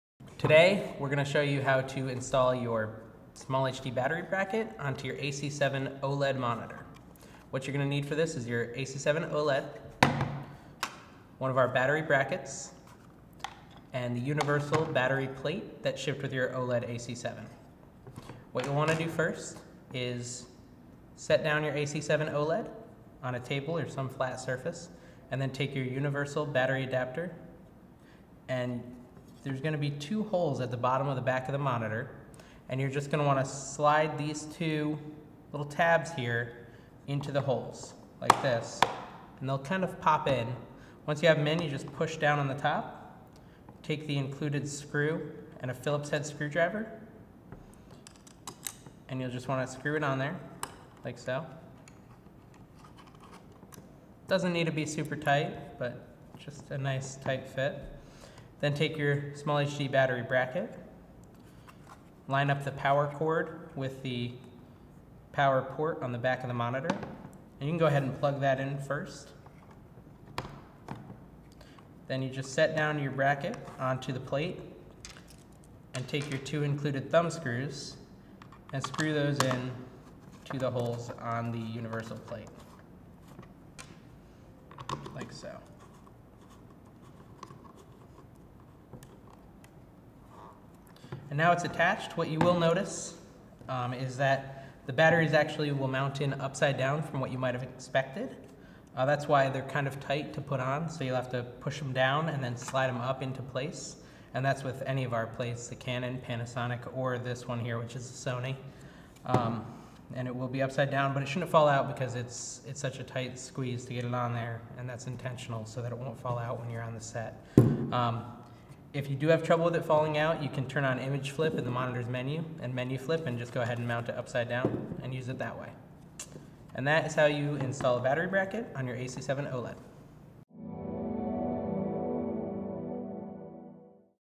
How to Install a SmallHD AC7-OLED Battery Bracket
In this video, we will show you how to install a battery bracket to the SmallHD AC7-OLED field monitor.  For more information about the AC7-OLED, please visit us on our